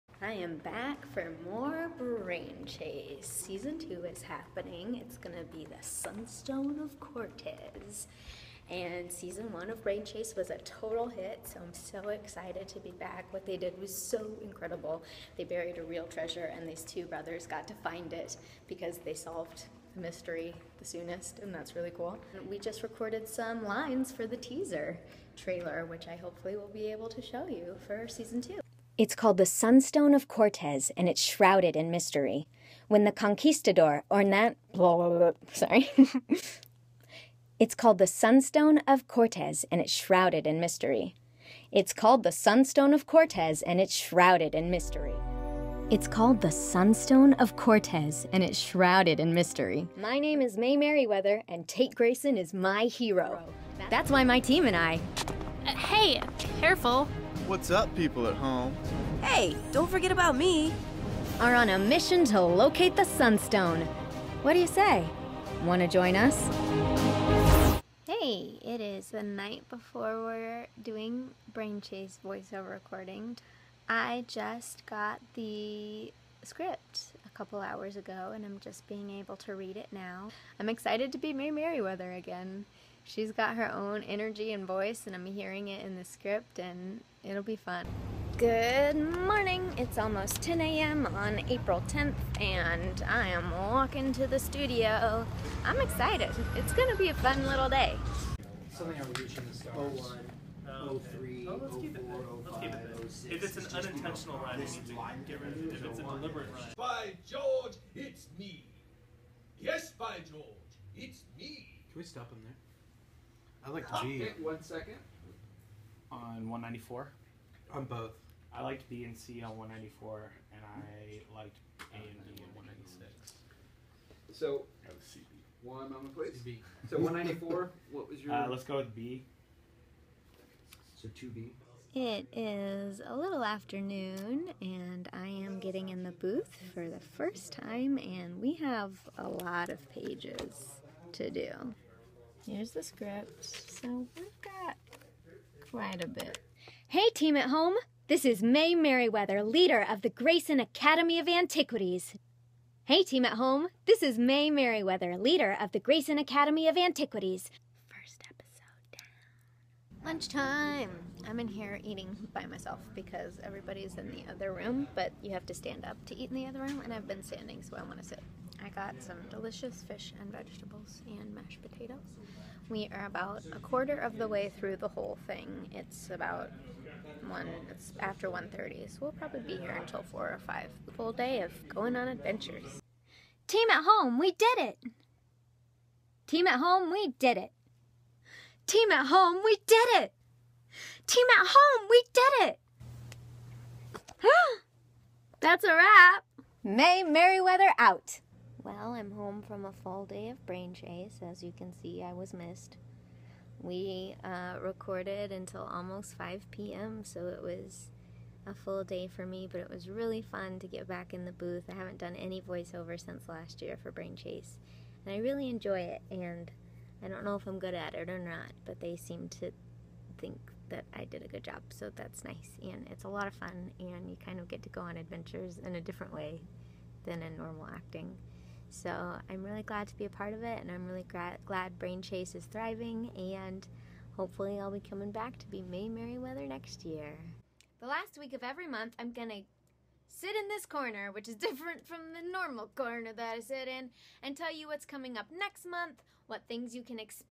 On Season 2 of Brain Chase!
The educational adventure program that I voice the lead character for, Brain Chase, is coming back for a second season this summer. Check out the process of recording the dialogue for Mae Merriweather!

Mike Knudsen
Cameron MacFarland
Brian Peterson
Stanislav Isotov
Kendra Lancaster
Chris Seekell
Jason J. Samuel
Kyle Wray
Mack Elder
Hannah Wharton
Brandi Hillier